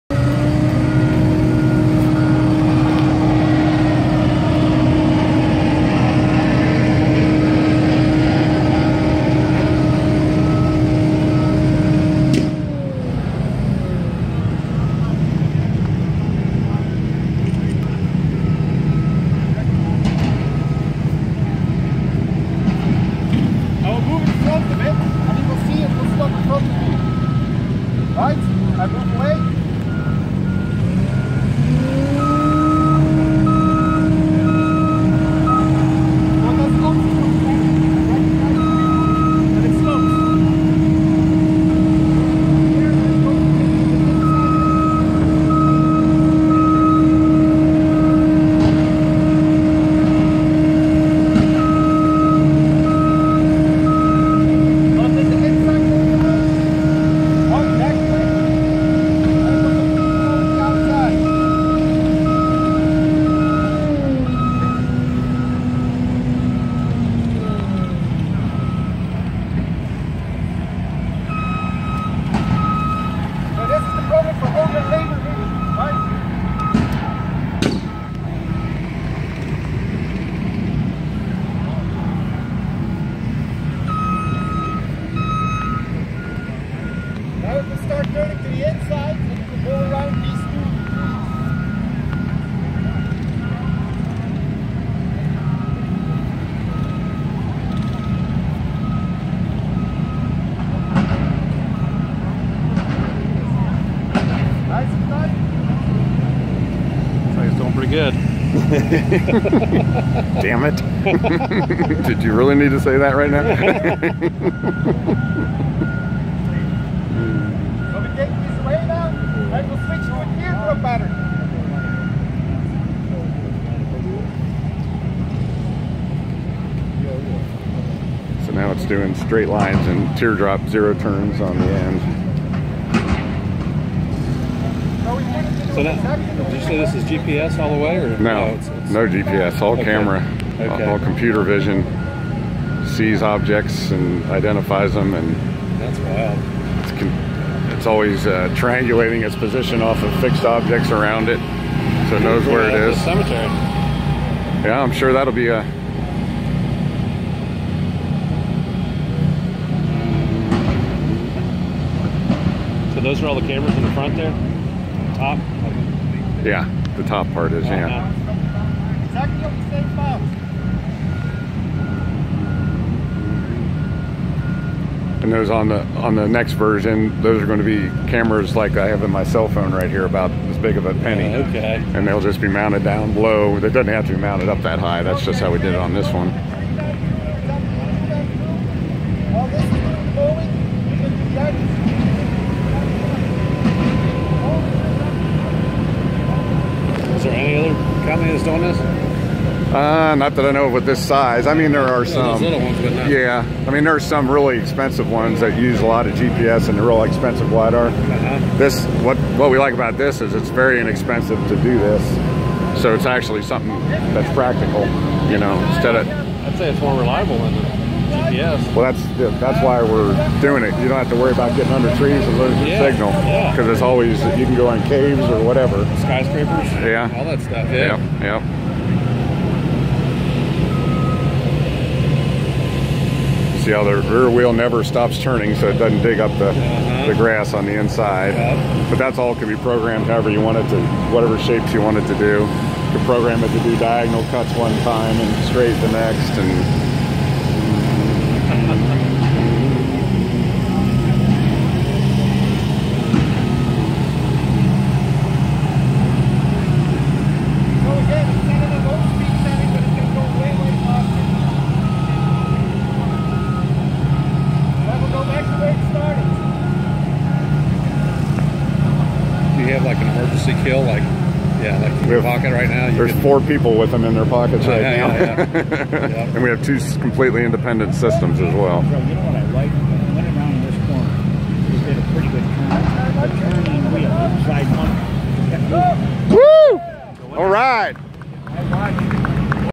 Kobi's computer vision technology working in Mean Green's 48" NXR ZT Mower.
This video is our demonstration at the GIE+ Expo in October 2019 in Louisville, Kentucky, of Mean Green's 48" NXR ZT Mower utilizing our computer vision technology. Our goal is to help landscapers with the current labor shortage problem.